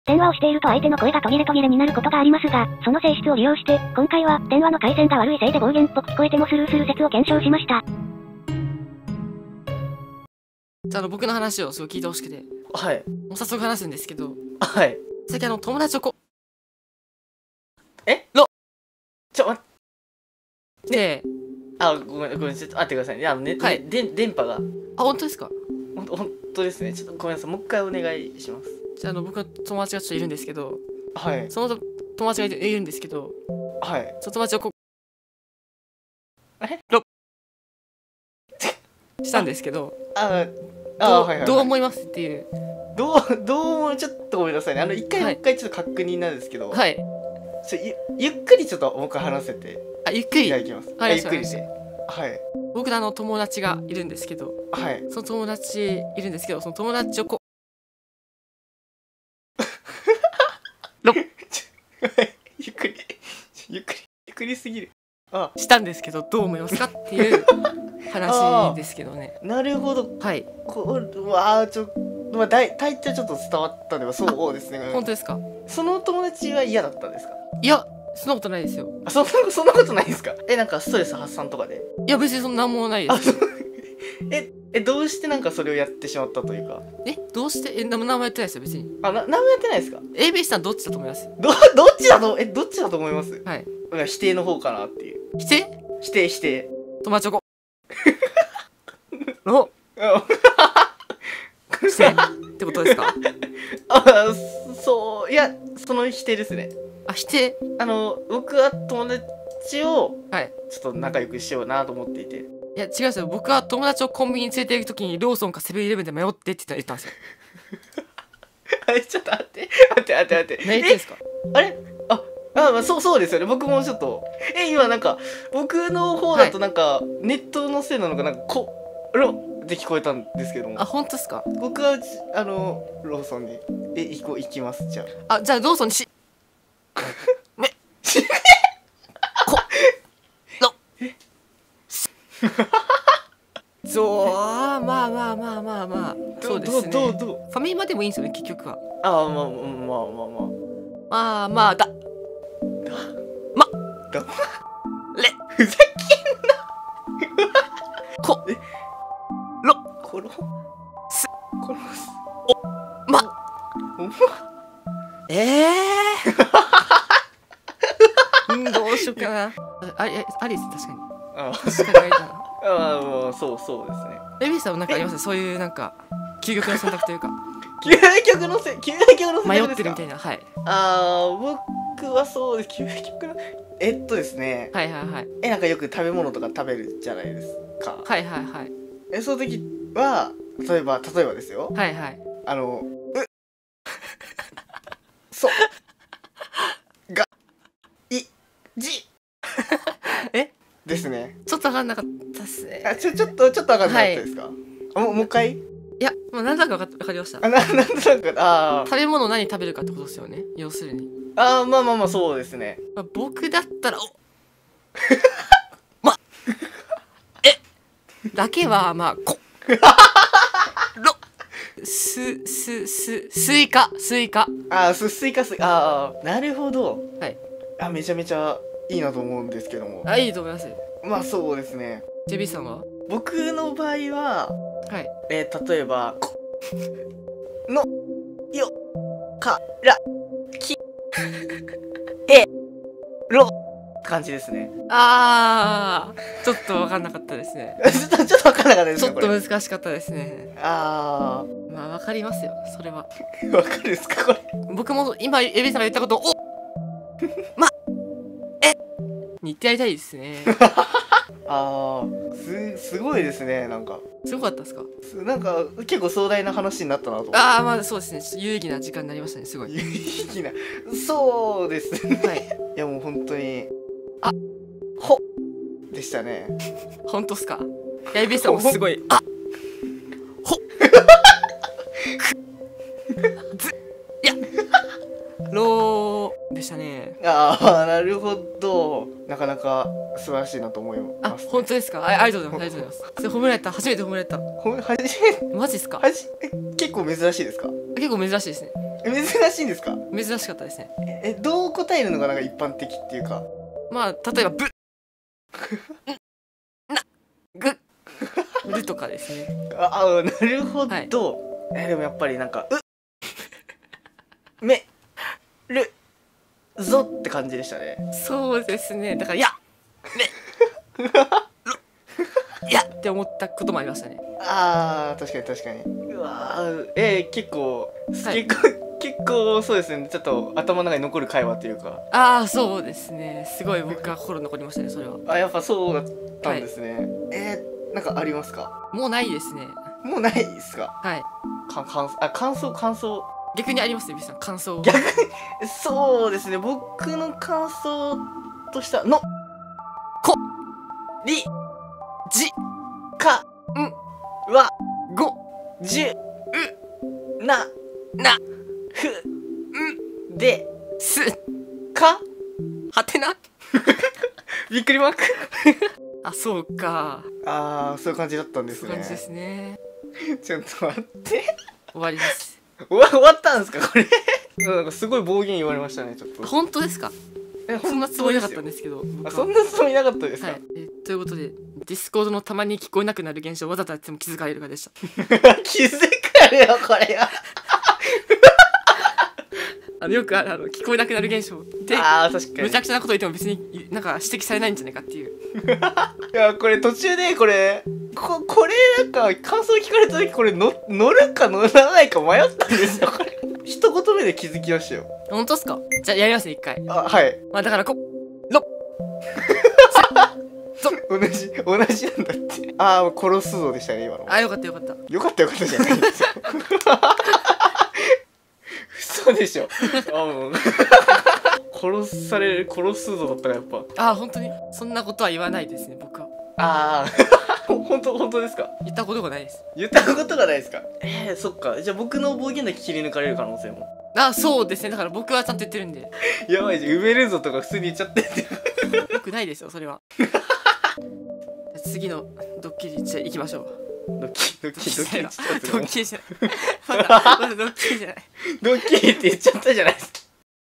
"電話の電波"が悪くて暴言っぽく聞こえてもスルーする説
90000人目標！！

■オススメ■